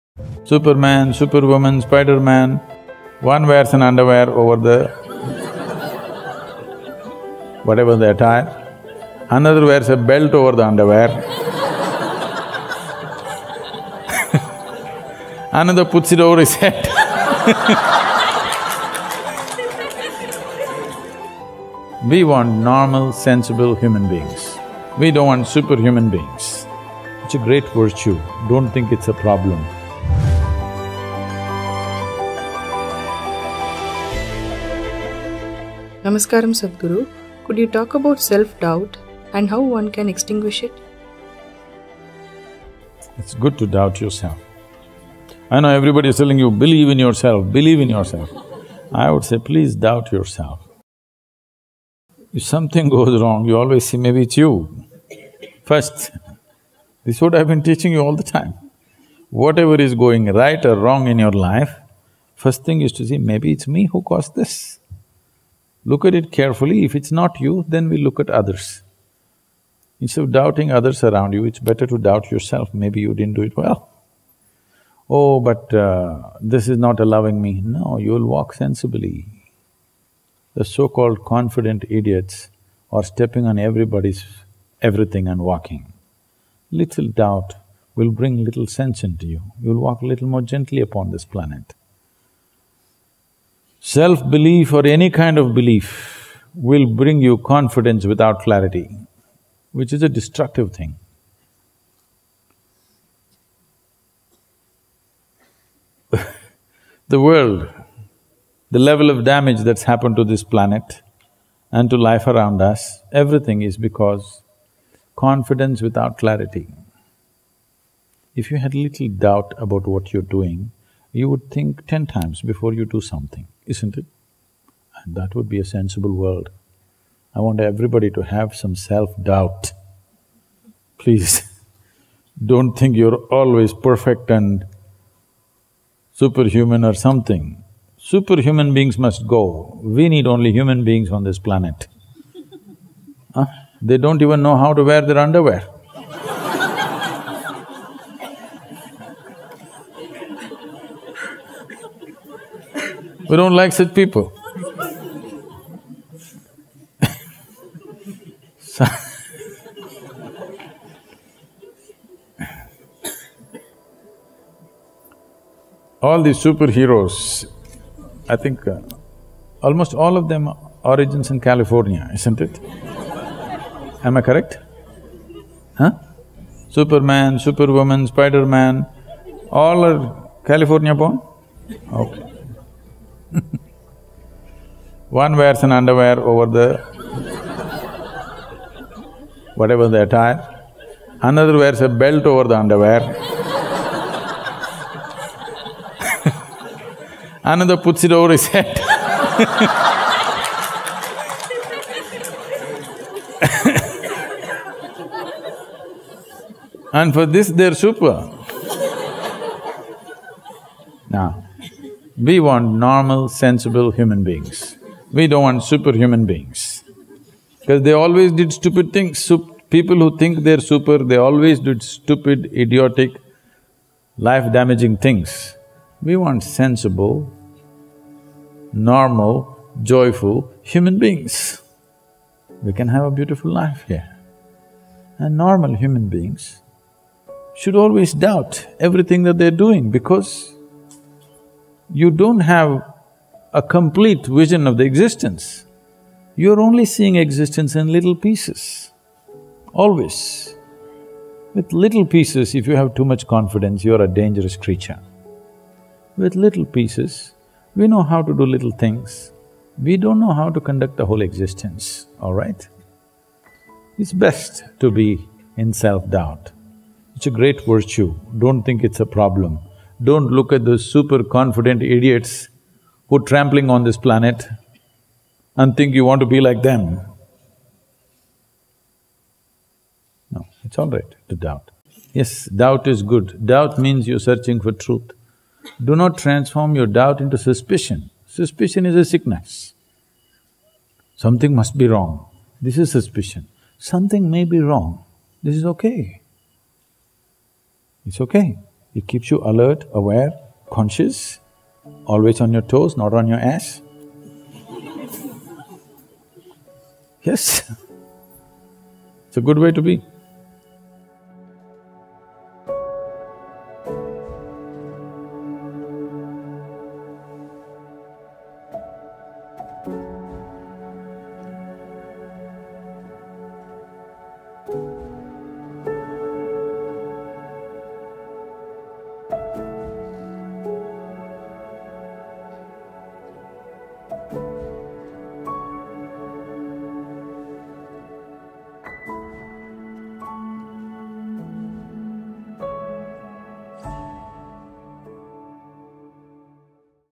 What Happens When You Doubt Yourself Sadhguru Answers - Message From Sadhguru
Sadhguru explains why self-doubt is a great virtue, whereas confidence without clarity could be destructive.